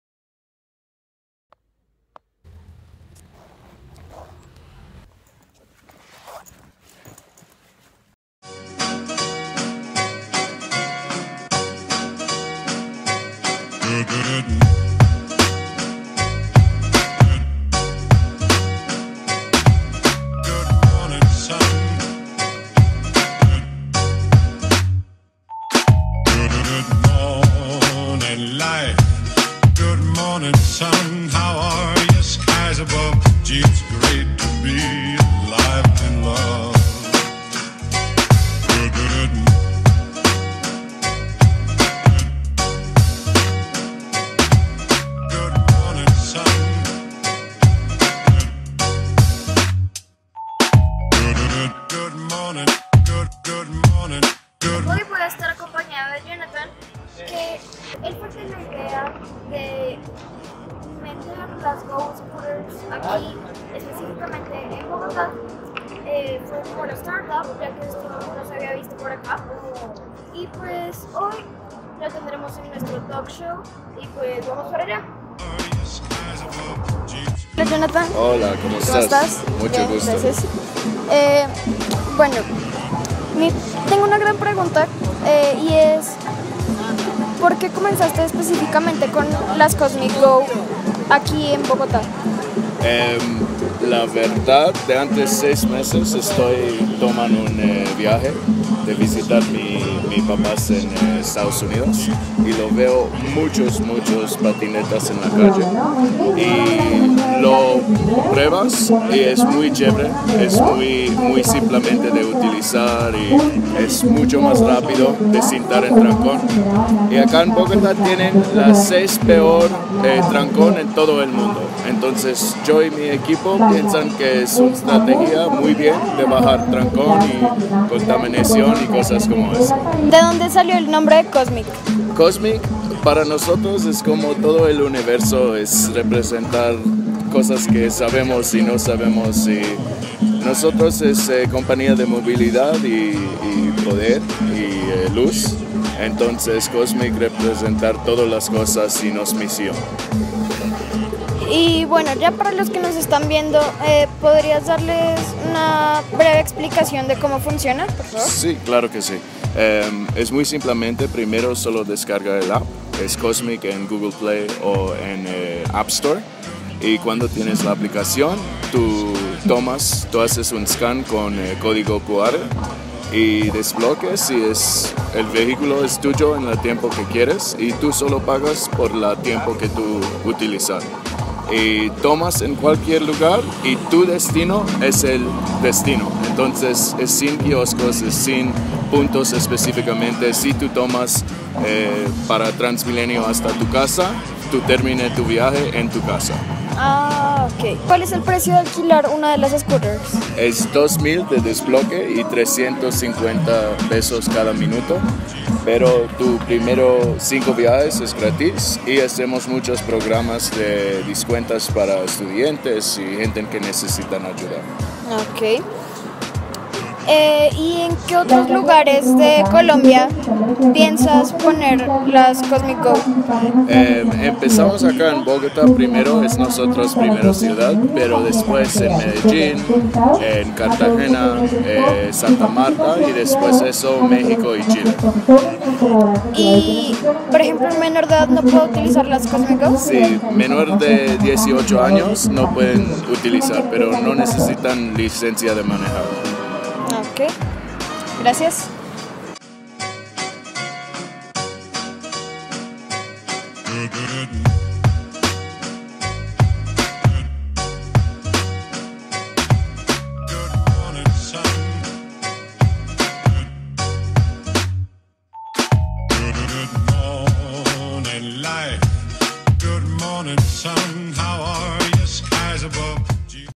Talk Show Ep.4 Cosmic go scooters en Bogotá (c/Jonathan Calmus)
Hola a todos!!! hoy traigo un Talk Show hablando má que nada de movilidad y actualidad con Jonathan Calmus, espero lo disfruten